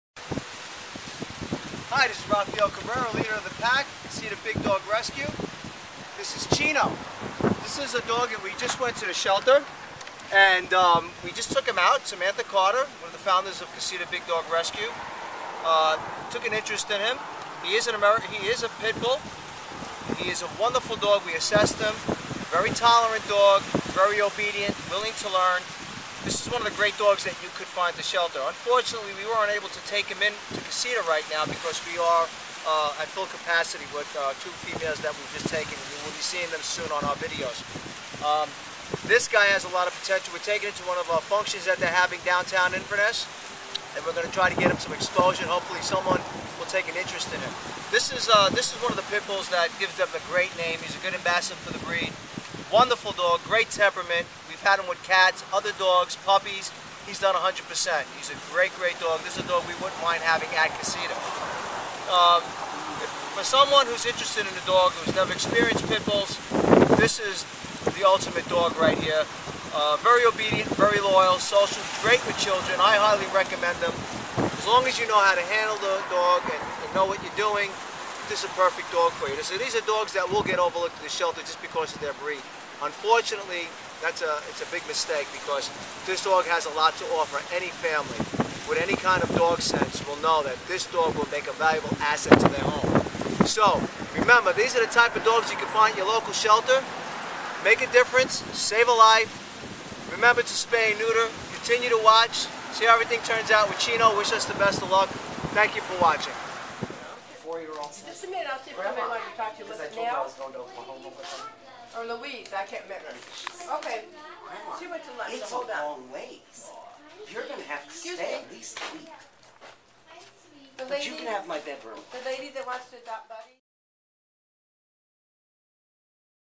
Casita Big Dog Rescue - SAVE A LIFE!
Leader of the Pack Rafael Cabrera introduces Chino, a Pit Mix from his local shelter - another example of a great dog you can find at your local shelter.

The GREAT news is Chino ended up getting adopted the day after this video was taken!